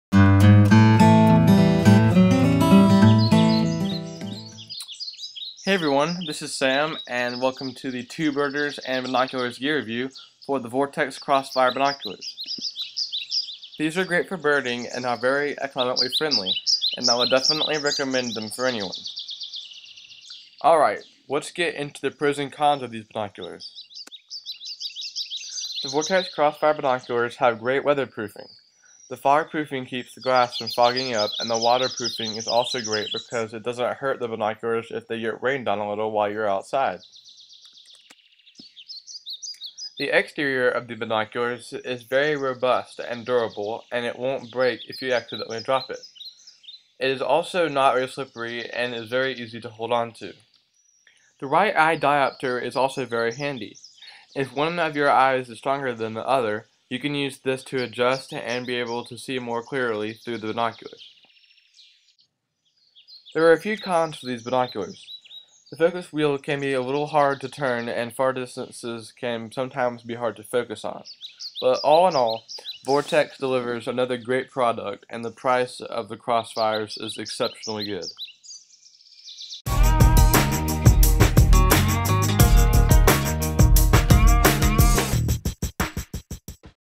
Gear Review: Vortex Crossfire 8x42
We review the Vortex Crossfire 8x42 Binoculars in this video.  These are a great pair of binoculars and we highly recommend them.